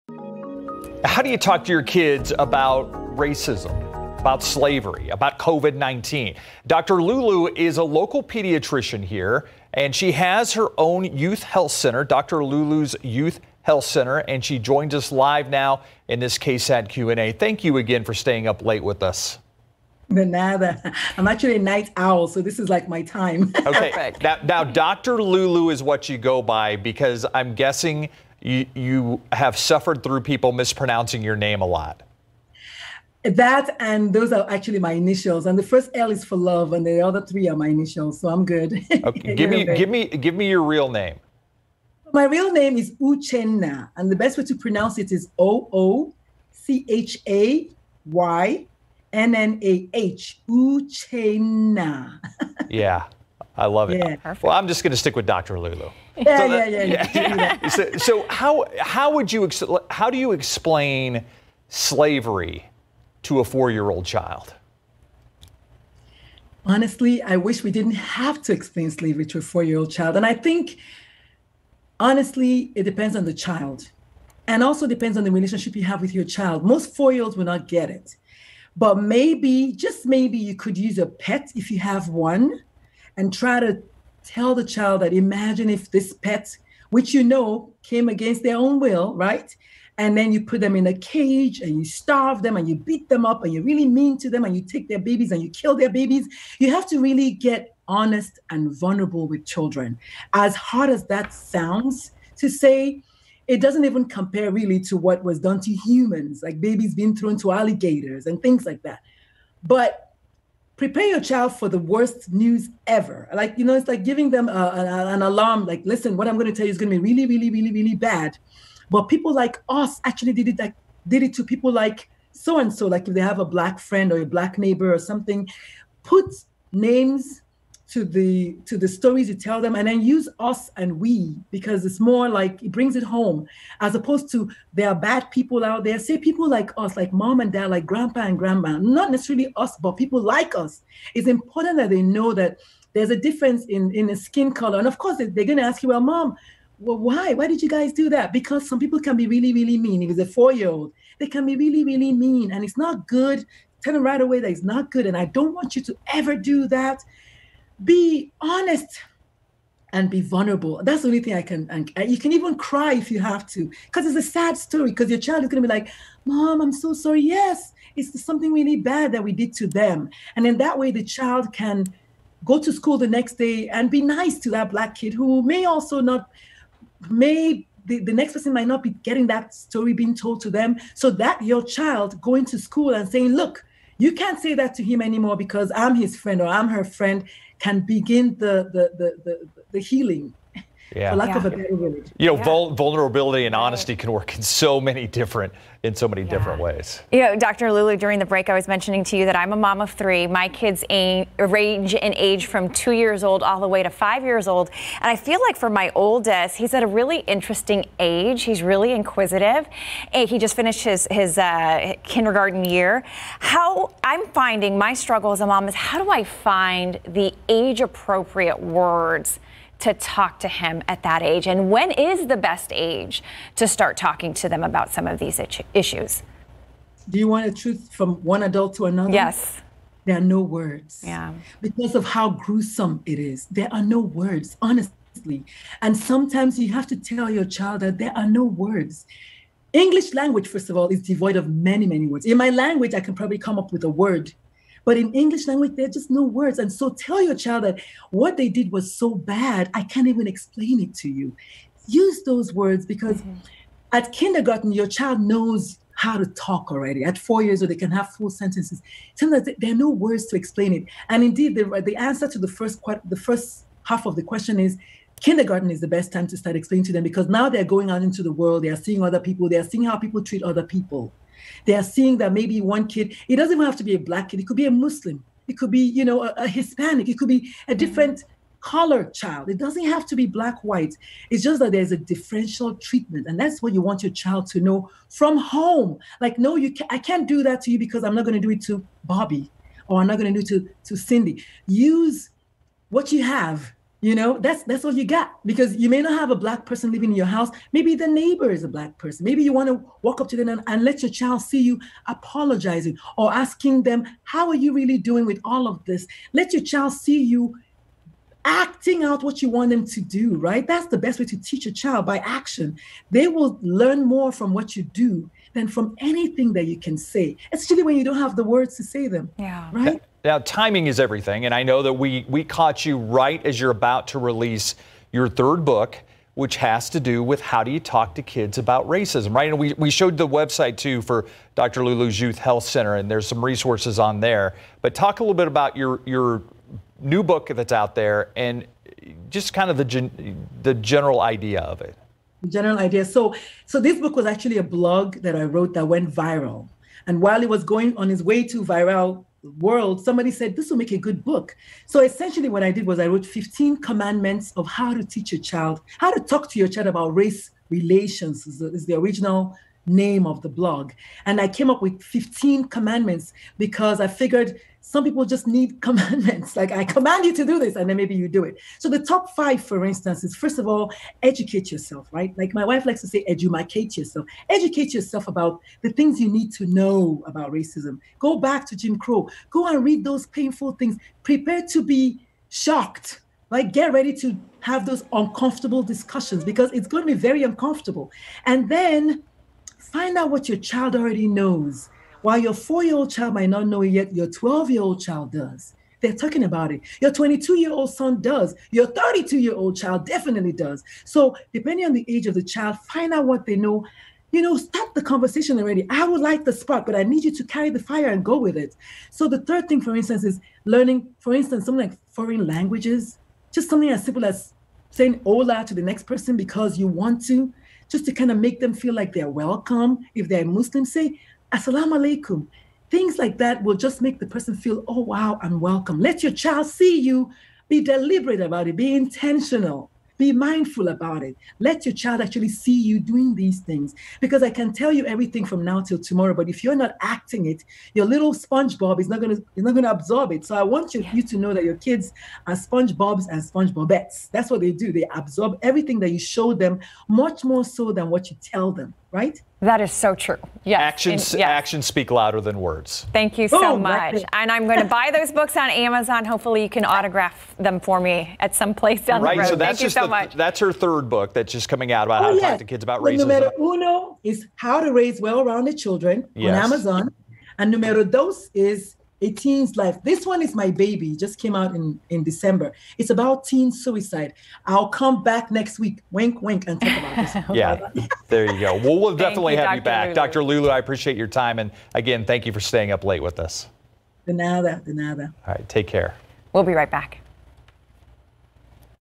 [10:43 PM] Brina Monterroza Title: KSAT Q&A: San Antonio pediatrician discusses how you can ...
Dr. Lulu discusses how you can tackle difficult conversations, from slavery to race relations, with your children